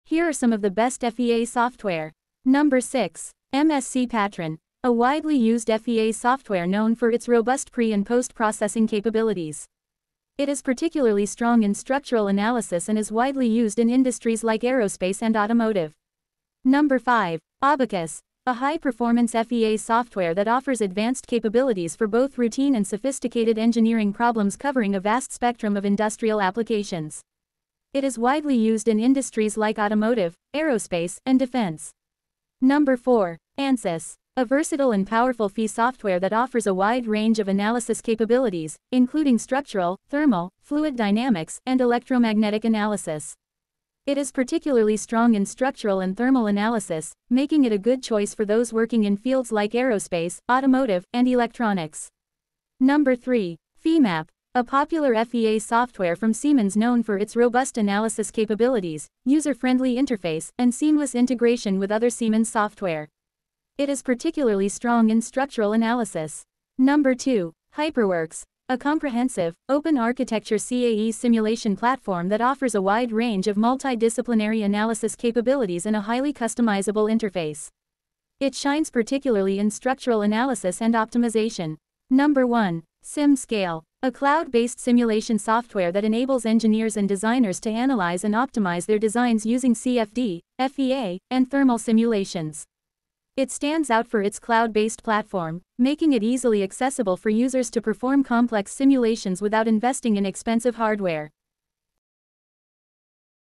Best FEA Software in 2024 for structural analysis | Finite element analysis for mechanical engineers
Here are some of the best FEA software ¹:

- *Msc Patran*: A widely used FEA software known for its robust pre- and post-processing capabilities. It is particularly strong in structural analysis and is widely used in industries like aerospace and automotive.

- *Abaqus*: A high-performance FEA software that offers advanced capabilities for both routine and sophisticated engineering problems covering a vast spectrum of industrial applications. It is widely used in industries like automotive, aerospace, and defense.

- *ANSYS*: A versatile and powerful FEA software that offers a wide range of analysis capabilities, including structural, thermal, fluid dynamics, and electromagnetic analysis. It is particularly strong in structural and thermal analysis, making it a good choice for those working in fields like aerospace, automotive, and electronics.

- *Femap*: A popular FEA software from Siemens known for its robust analysis capabilities, user-friendly interface, and seamless integration with other Siemens software. It is particularly strong in structural analysis.

- *HyperWorks*: A comprehensive, open-architecture CAE simulation platform that offers a wide range of multi-disciplinary analysis capabilities and a highly customizable interface. It shines particularly in structural analysis and optimization.

- *SimScale*: A cloud-based simulation software that enables engineers and designers to analyze and optimize their designs using CFD, FEA, and thermal simulations. It stands out for its cloud-based platform, making it easily accessible for users to perform complex simulations without investing in expensive hardware.